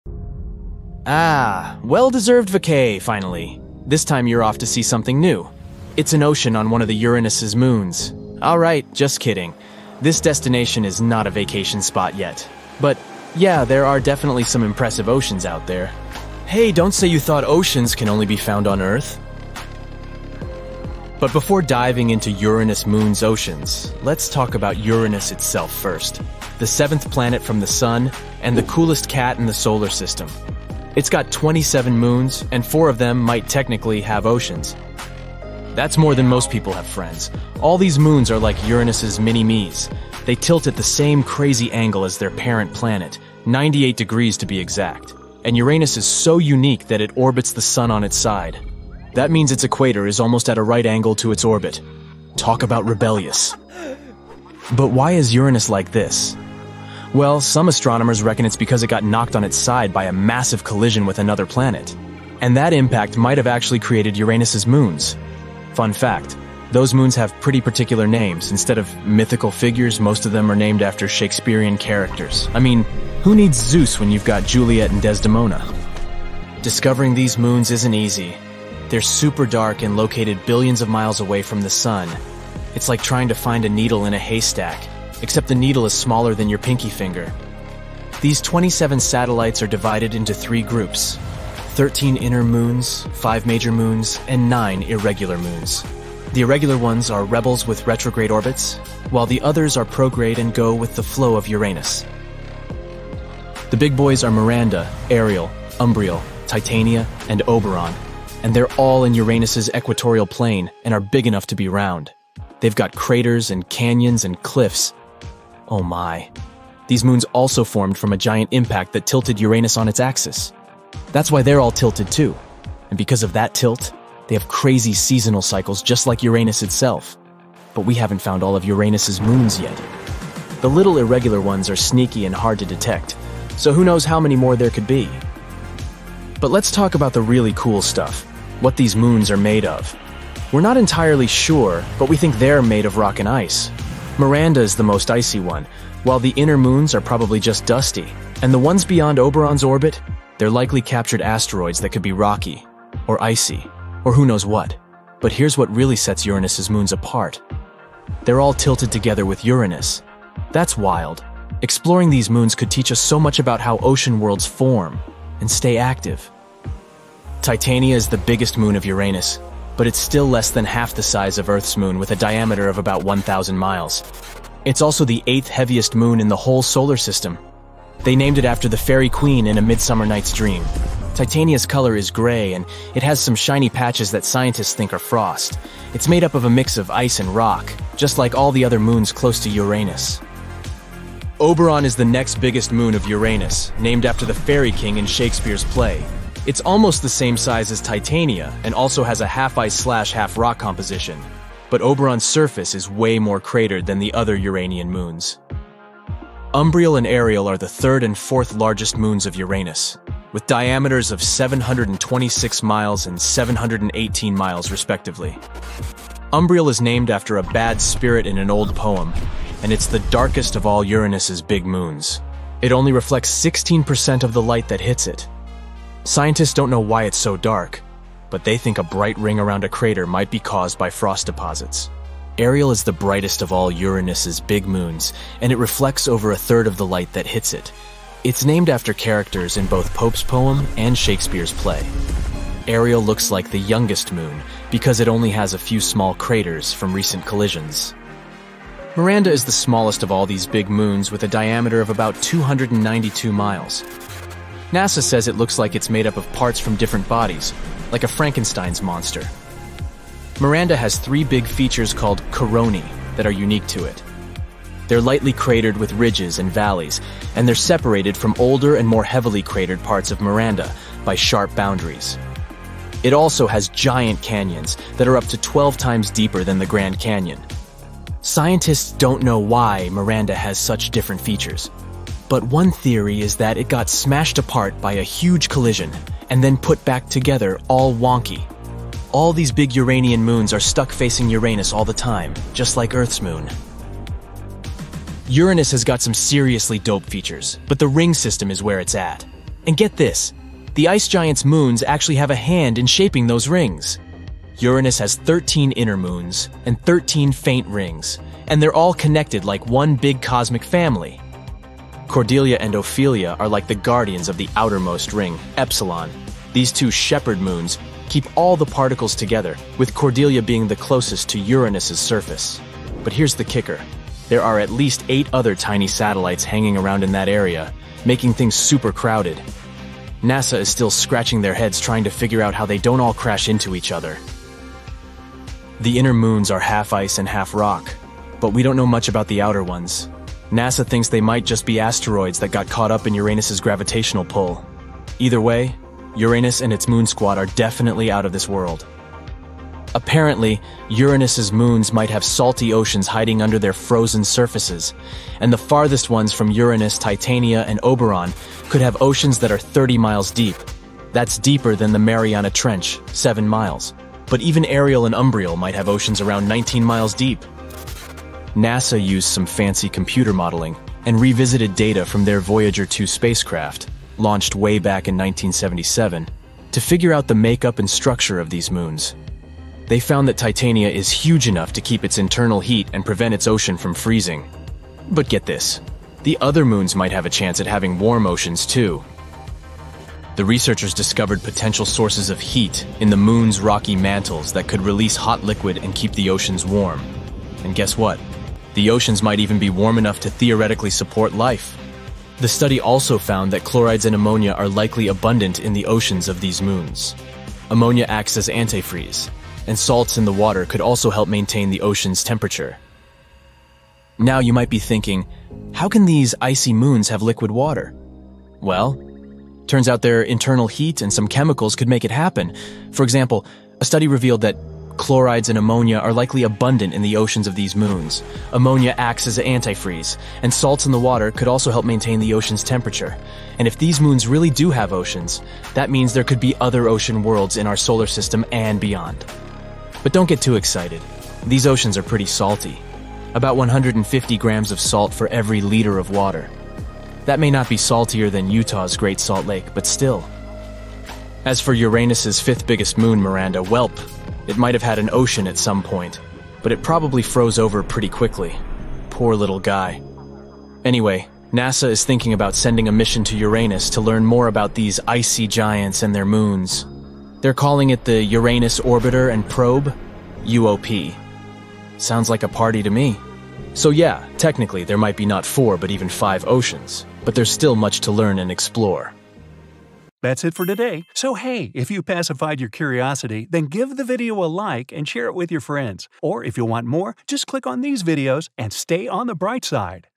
Life Could Be Hiding on Four Moons of Uranus
Uranus has 27 moons, with four potentially having oceans. These moons were formed from a collision that tilted Uranus on its side. The major moons, including Titania, Oberon, Umbriel, Ariel, and Miranda, exhibit diverse features and are made of rock and ice. Uranus' moons contribute to shaping its ring system. Recent research suggests that these moons could have salty oceans, potentially supporting life. Further exploration is being considered through the Uranus Orbiter and Probe (UOP) mission.